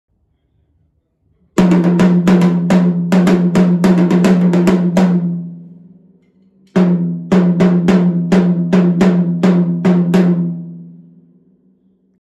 Basix dětský pochodový bubínek 10" x 4"
Pochodový bubínek vhodný pro děti, velikost 10" x 4" ( 25 x 10 cm). Více informací a prodej zde